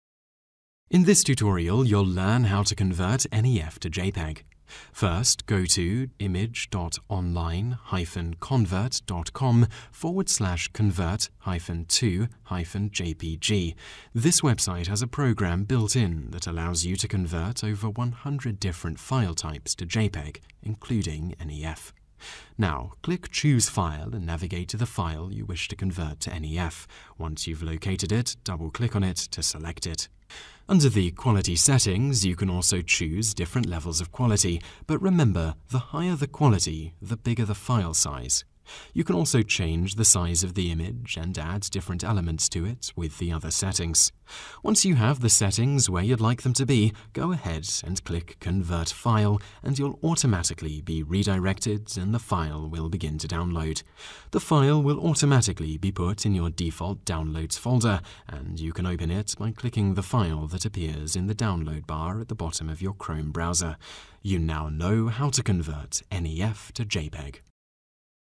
How to Convert NEF to JPEG
This tutorial will show you how to convert a NEF file to a JPEG.

In this tutorial, you'll learn how to convert NEF to JPEG.

Step # 1 -- Navigating to the online program

First, go to " this website has a program built in that allows you to convert over 100 different file types to JPEG, including NEF.

Step # 2 -- Uploading the NEF

Now, click "Choose File" and navigate to the file you wish to convert to NEF, once you've located it double-click it to select it.

Step # 3 -- Editing the settings

Under the "Quality Settings" you can choose different levels of quality, but remember the higher the quality, the bigger the file size. You can also change the size of the image, and add different effects to it with the other settings.

Step # 4 -- Converting the file

The file will automatically be put into your default downloads folder, and you can open it by clicking the file that appears in the downloads bar at the bottom of your Chrome browser. You now know how to convert NEF to JPEG.